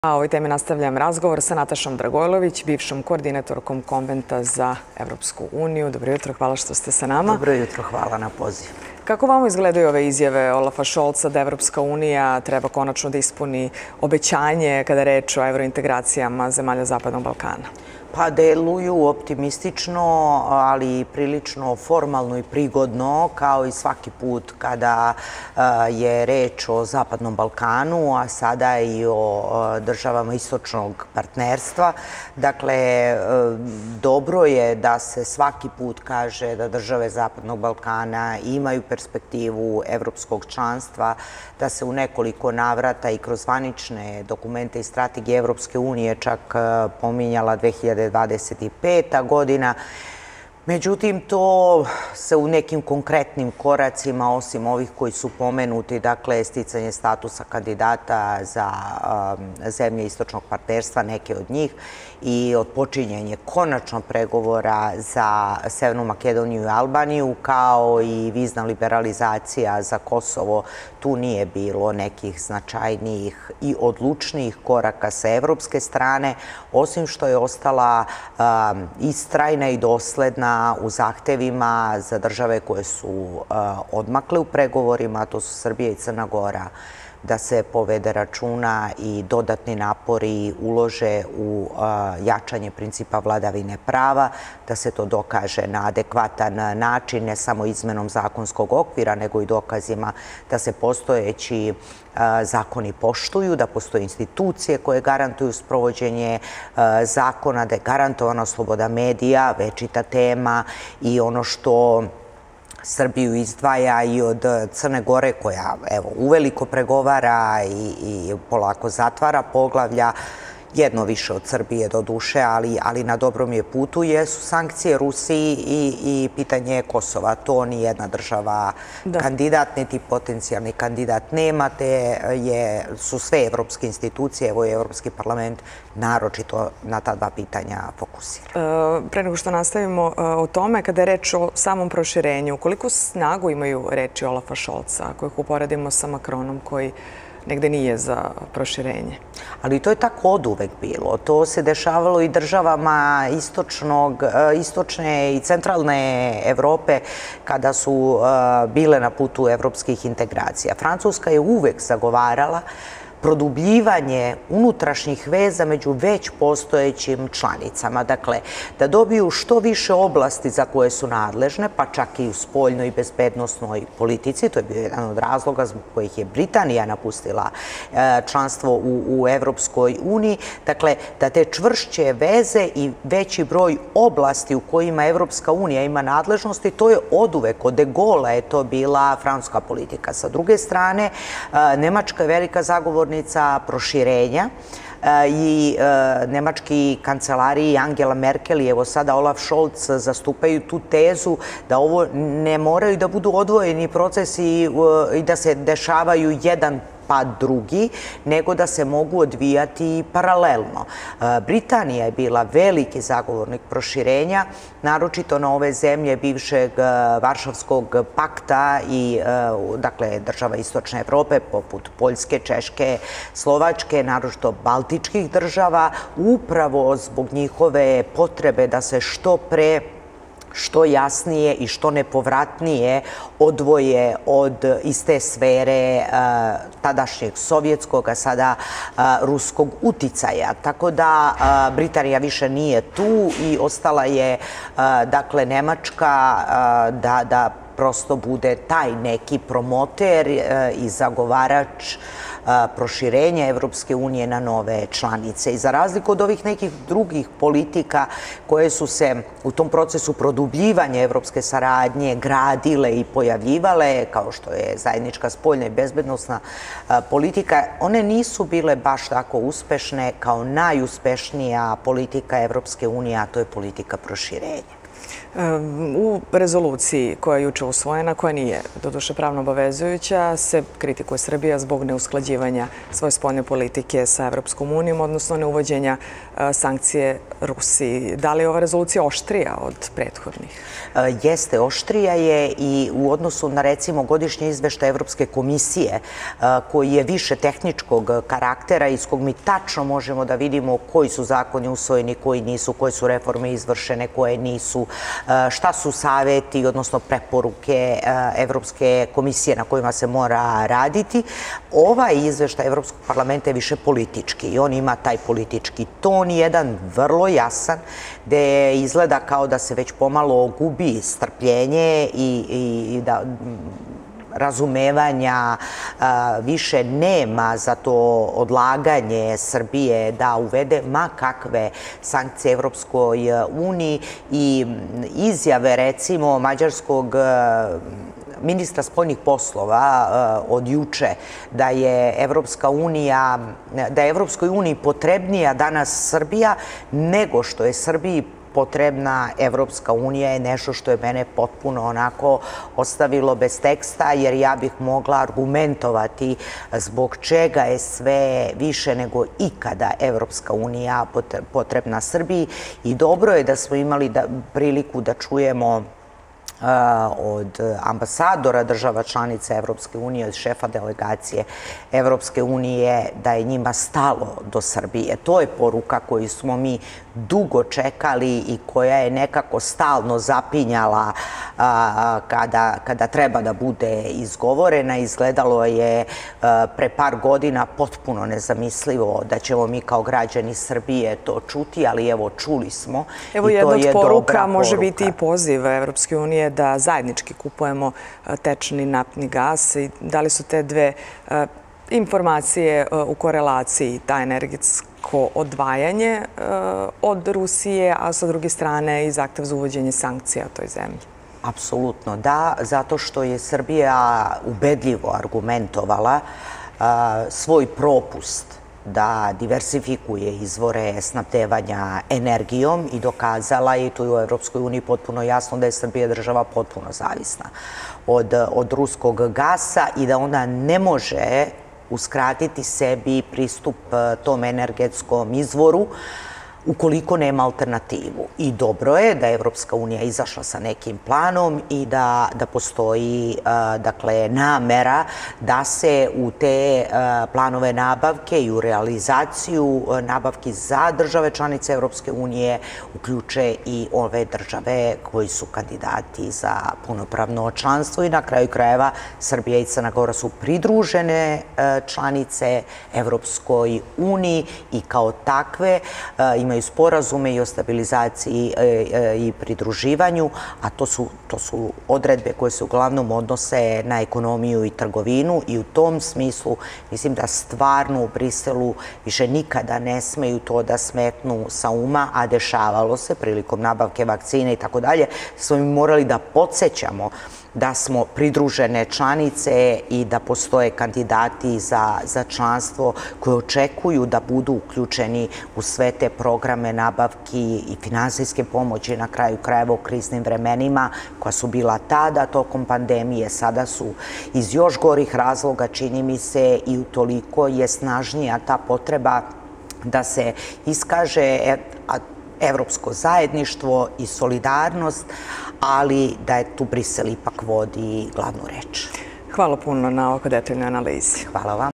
Dragojlović: Evropskoj uniji je stalo do Srbije
Nekadašnja koordinatorka Nacionalnog konventa o Evropskoj uniji Nataša Dragojlović kaže da je dobro da se svaki put ponovi da države Zapadnog Balkana imaju perpsektivu u proširenju Unije. Prema njenom mišljenju, ovogodišnji izveštaj Evropskog parlamenta o Srbiji oštriji je nego do sada.